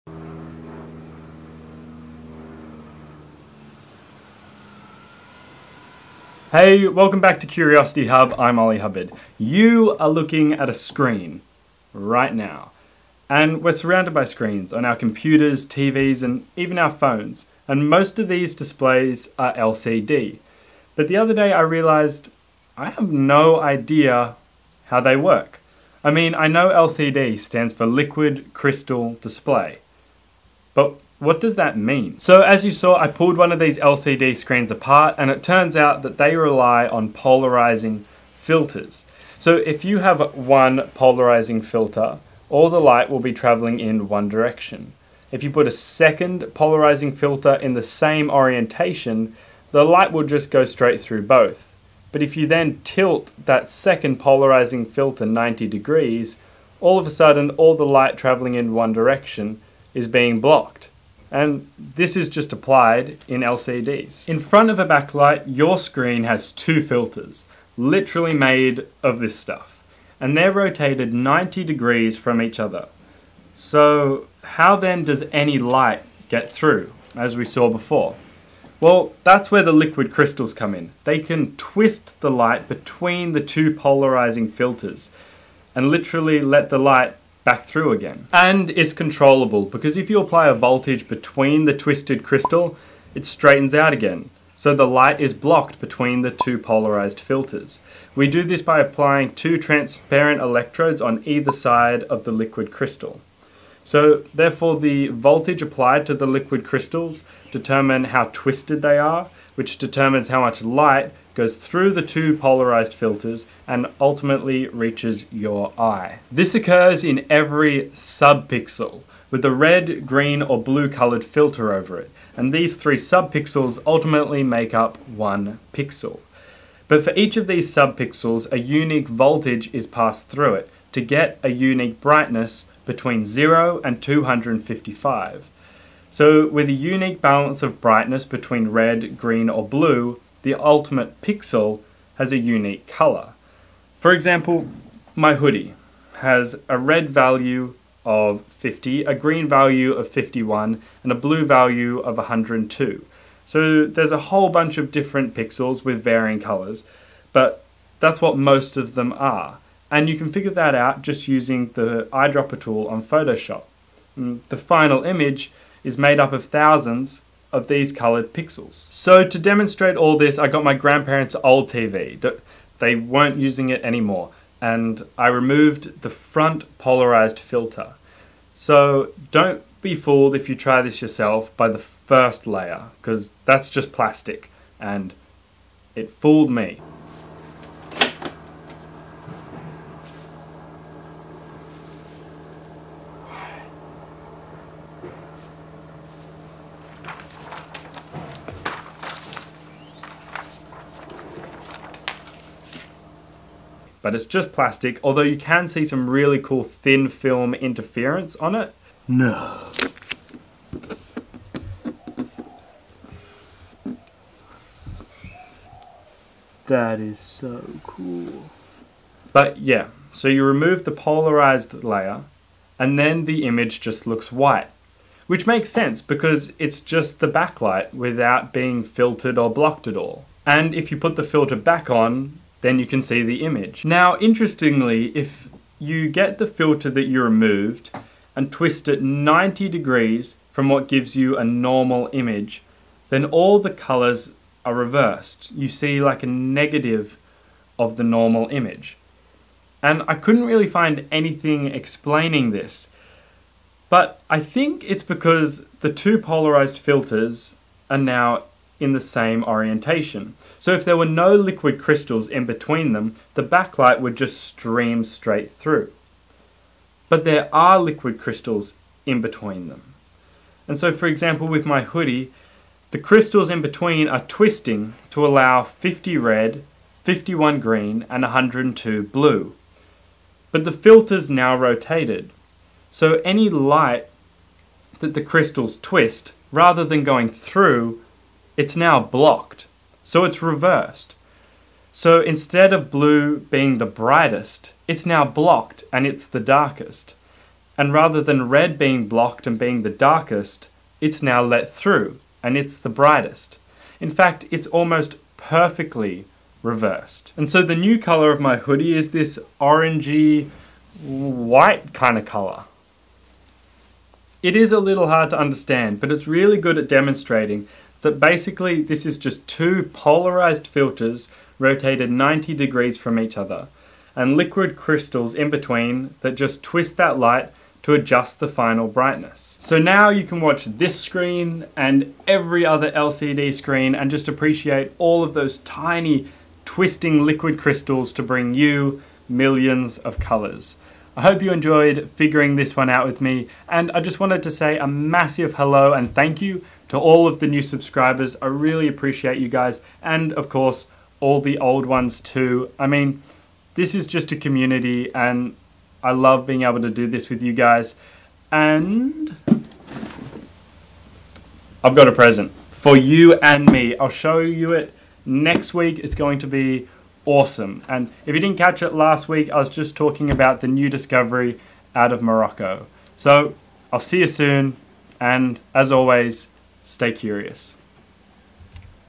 How LCD Works (LIQUID CRYSTALS)
This episode explores the technology behind the LCD screen your watching right now and how it works. The polarising filters and liquid crystals are explained for each sub-pixel, and how they form a coloured image.
You can also see how to remove the polarising filter and reveal the image (or invert it) at your own will.

QUESTIONS BELOW FOR FUTURE EPISODES!

Warm Regards,
Oli